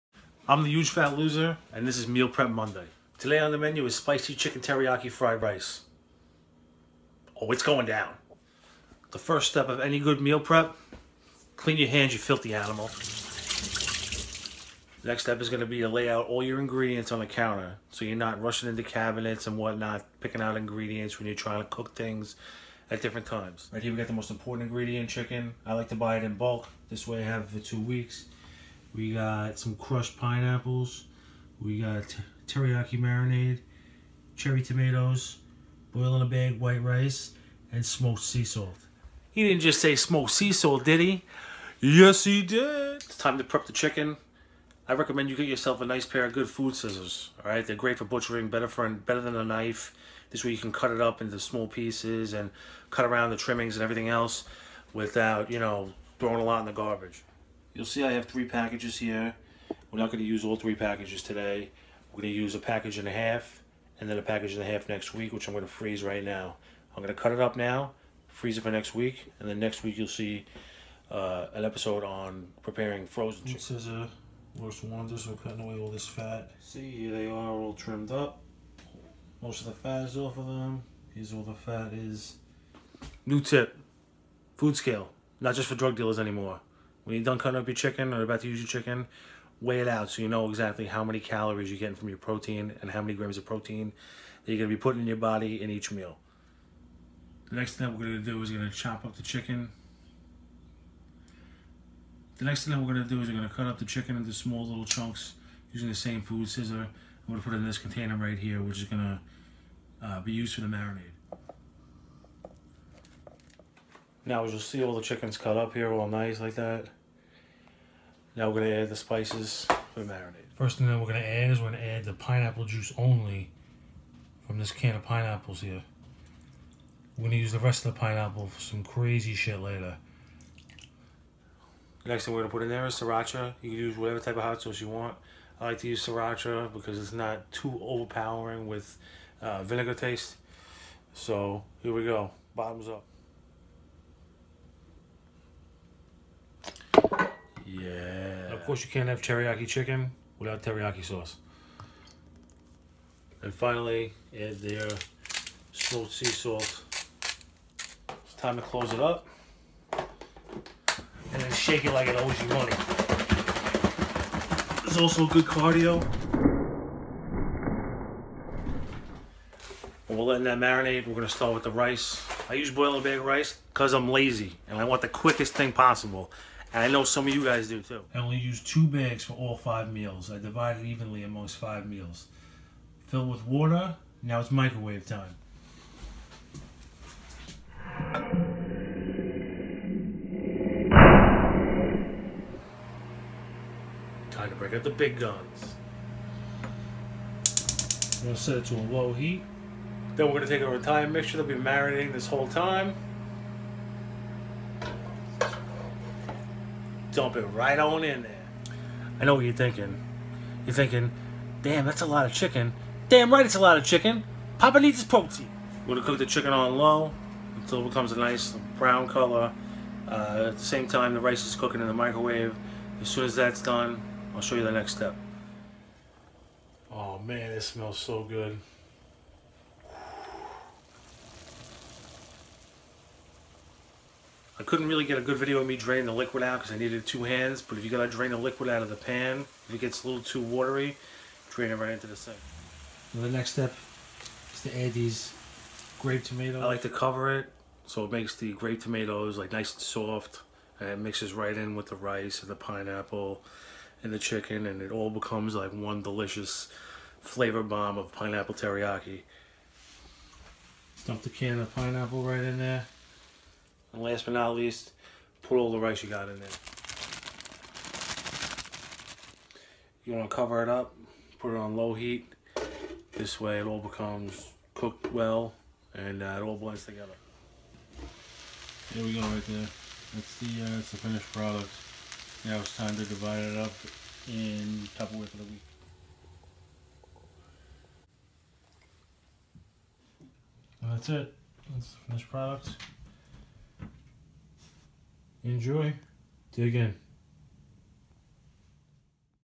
Meal Prep Monday: Volume 1 Blazin Pineapple Chicken Teriyaki Fried Rice
Here's volume 1 of Meal Prep Monday. 5 meals for the week, 1 dish. Ingredients list: Chicken Breast, smoked sea salt, crushed pineapple in pineapple juice, teriyaki sauce, hot sauce (sriracha), grape tomatoes and boil in a bag rice.